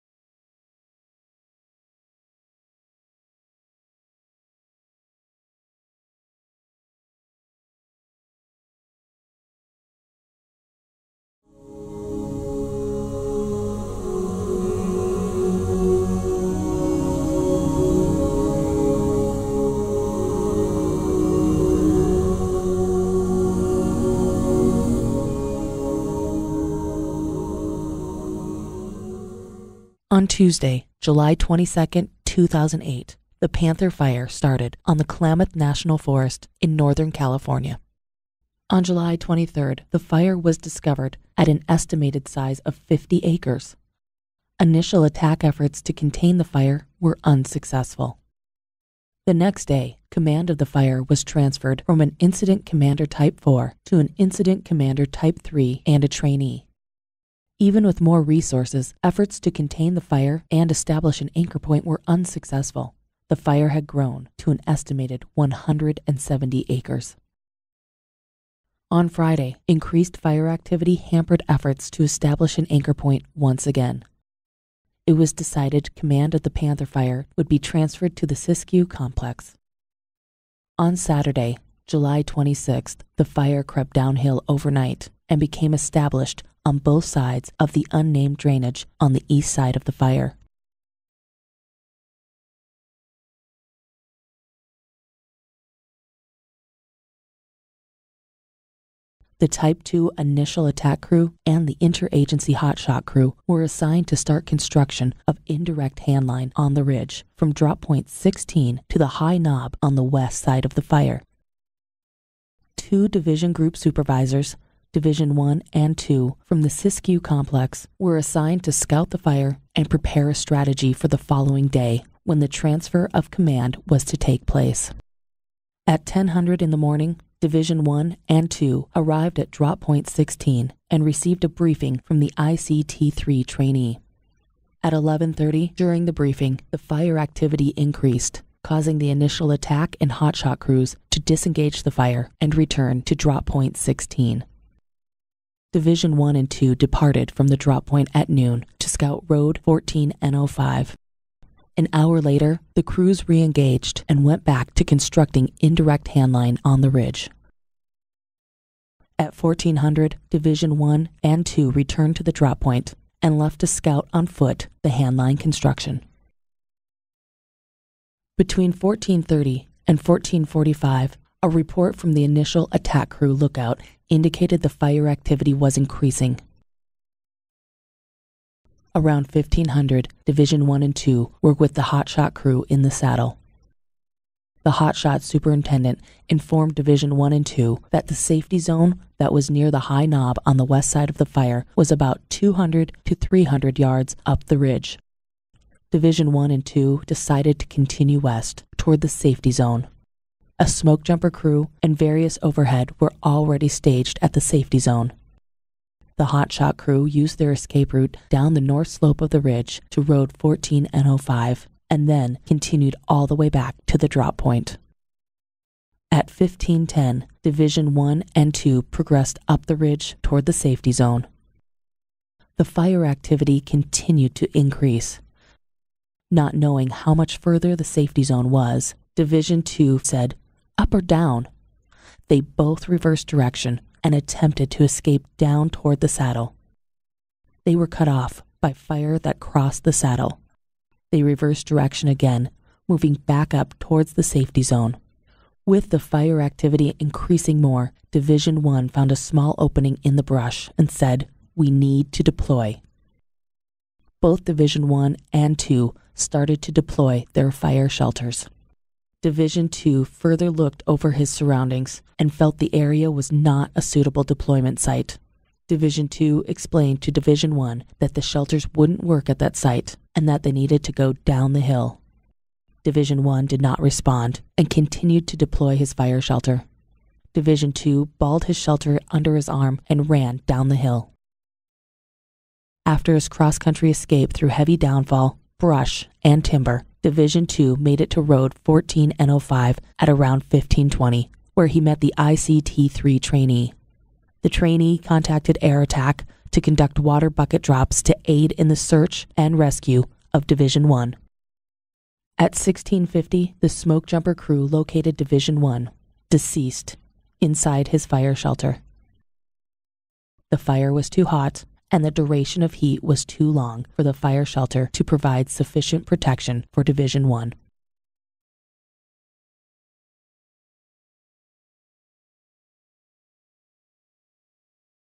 Panther Fire
"Fire Shelter Deployments: Stories and Common Insights" is a program developed by the US Forest Service Missoula Technology and Development Center (MTDC) that will help you understand what you may experience in a fire shelter deployment. For additional fire shelter information